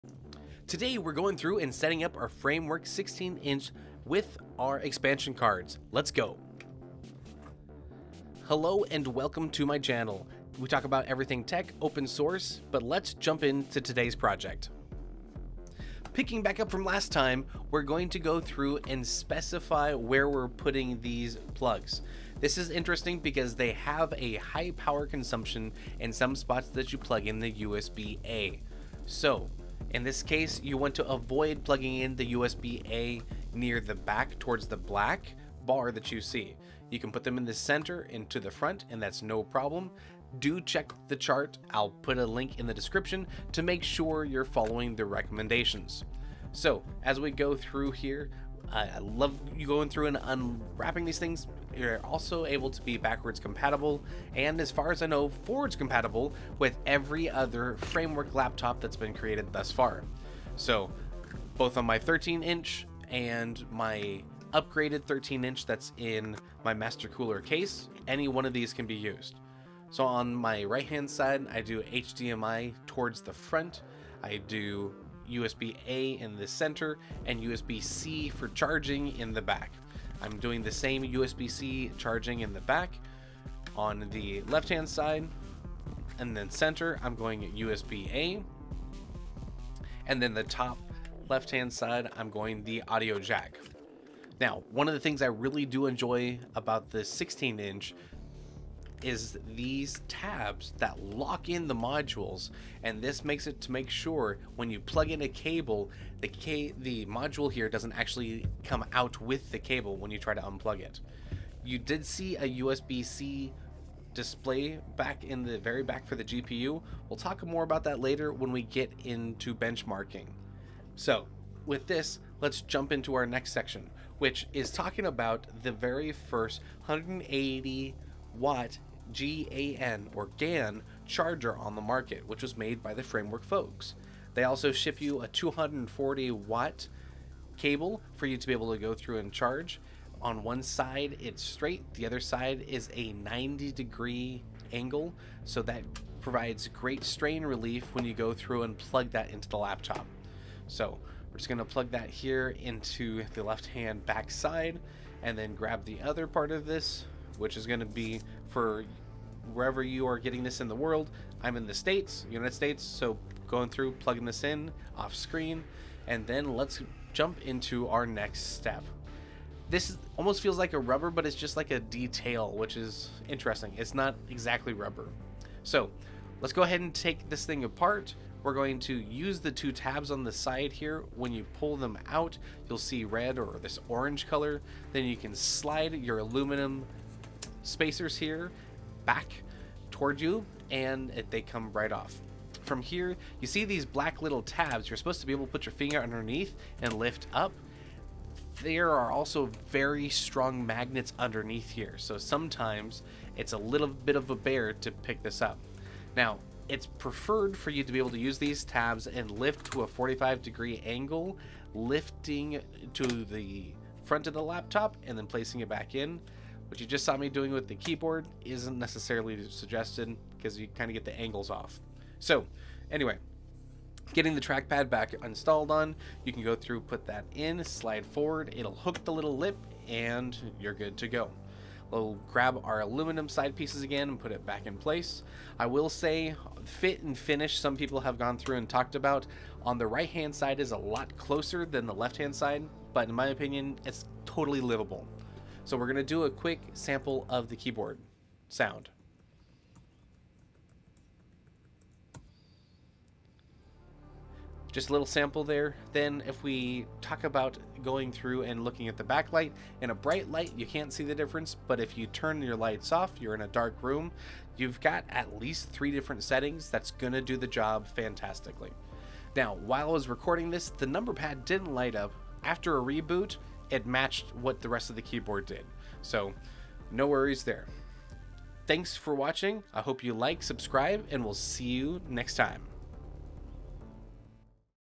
Framework 16 Expansion Cards
This is the Third of many Framework 16 Videos

Here is my blog post of my Favorite Framework Laptop Accessories

If you are interested in picking up a Framework Laptop check out the DIY kit.